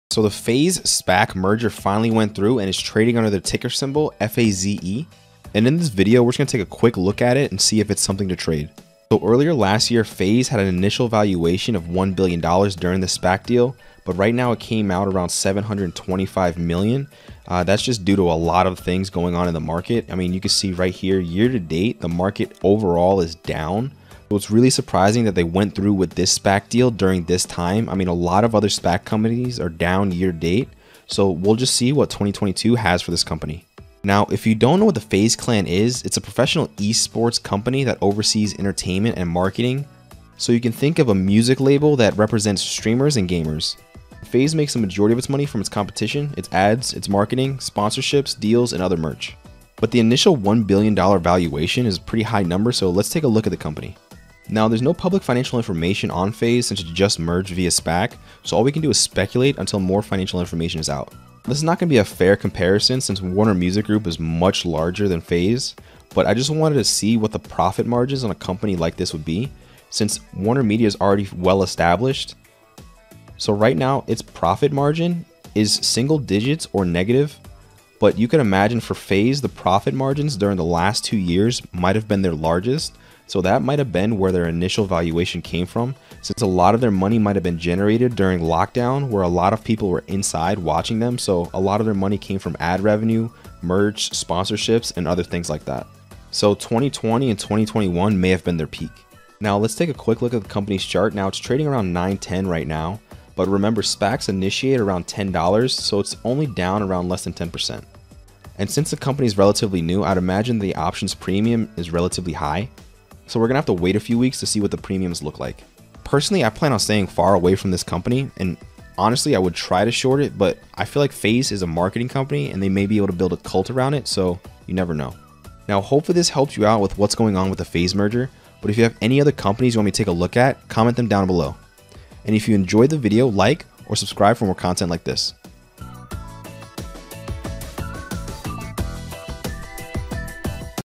Investing In FAZE Stock | You Need To Know!
FaZe Clan goes public in $725 million SPAC under ticker $FAZE. In this video I take a look a $FAZE.